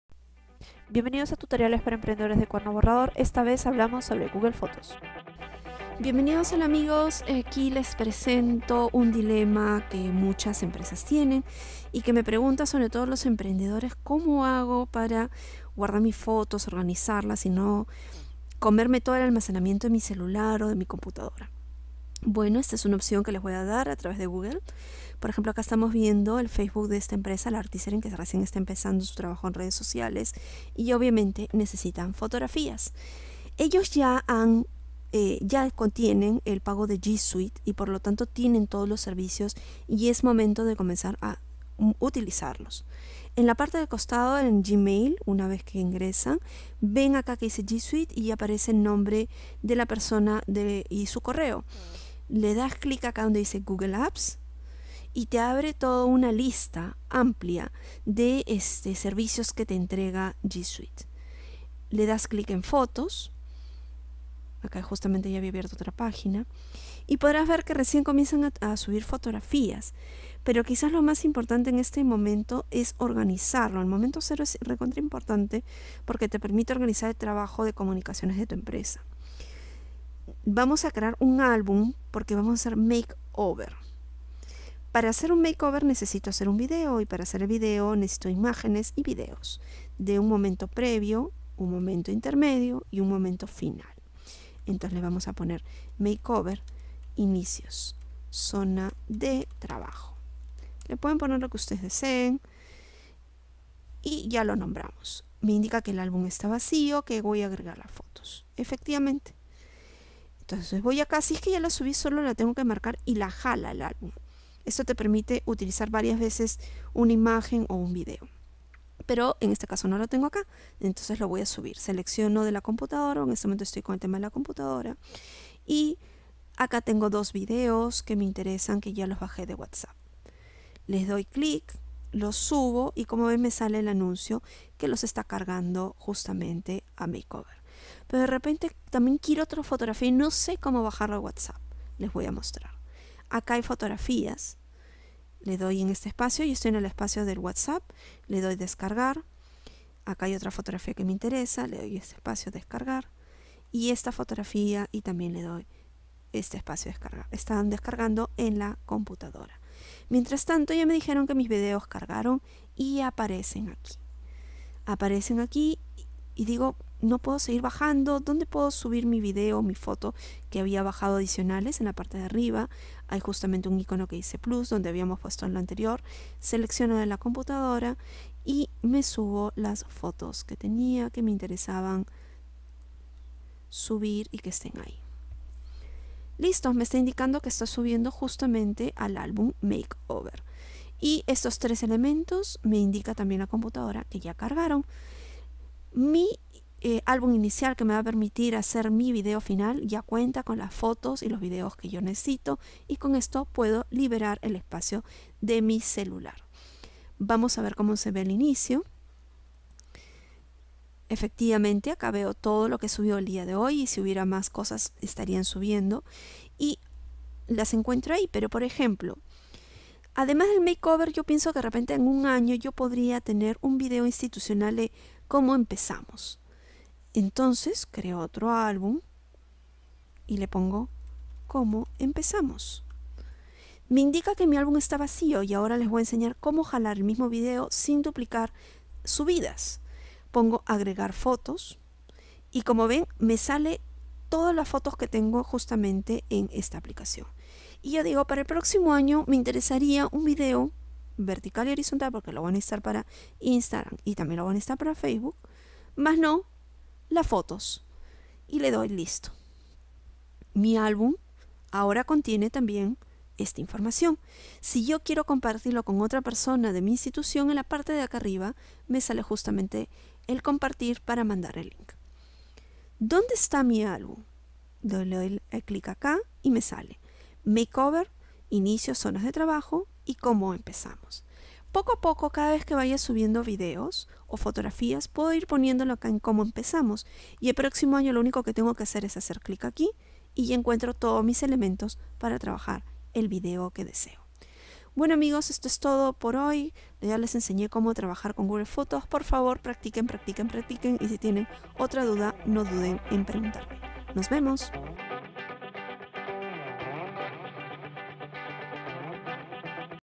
Tutoriales ¿Cómo usar GoogleFotos? ¿Cómo crear con un presupuesto mínimo?
¿Cómo va ahorrar espacio en las computadoras y celulares sin perder las fotos y videos que tienes y que consideramos estratégicas? No las borres, utiliza